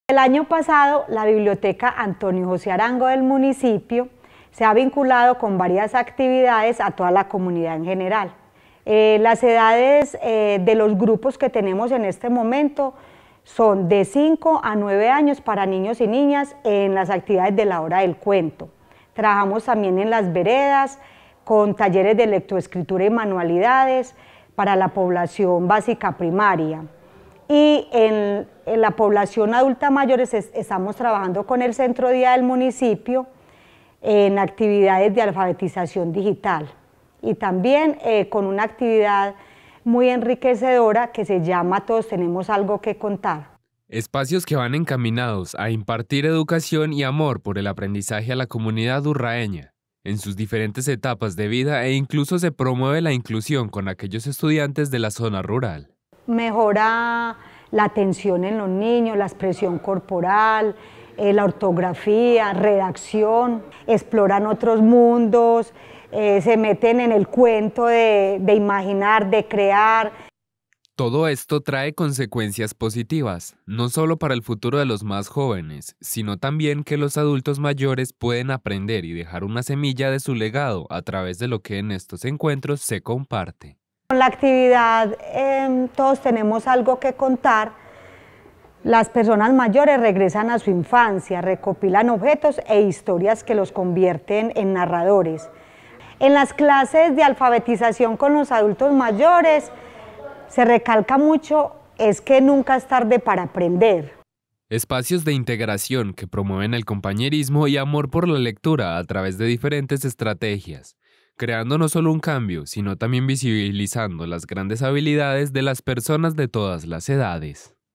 Talleres de la biblioteca Antonio José Arango
Talleres de lectoescritura, la hora del cuento, entre otros talleres, son impartidos desde la biblioteca Antonio José Arango, donde es de resaltar que todo tipo de población se vincula, incluyendo al adulto mayor. Eliana Sepúlveda, quien lidera estos procesos nos comparte en qué consisten y cuál es su principal objetivo